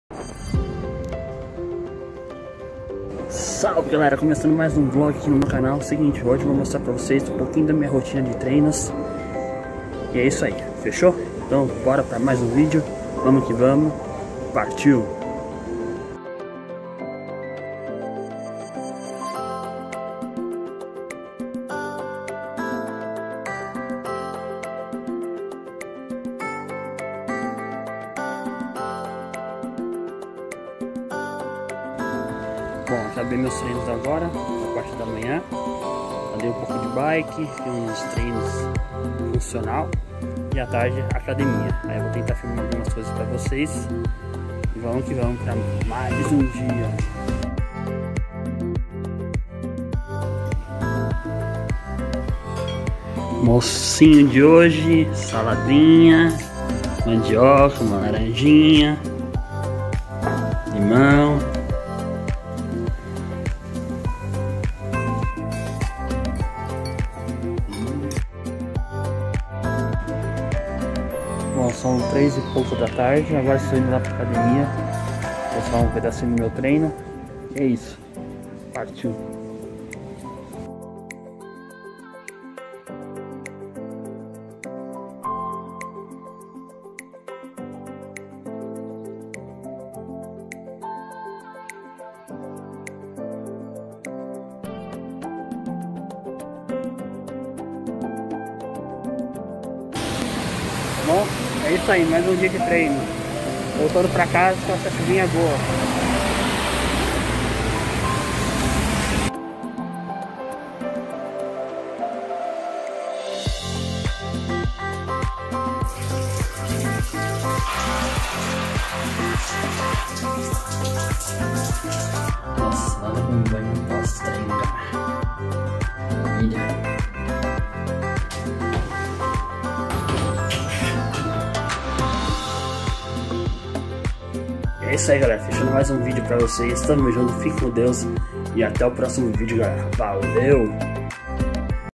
Mostrando minha rotina de treino pós festas ❤🎧🇧🇷🤟🎥
Pequeno registro de mais um dia de treino para vocês.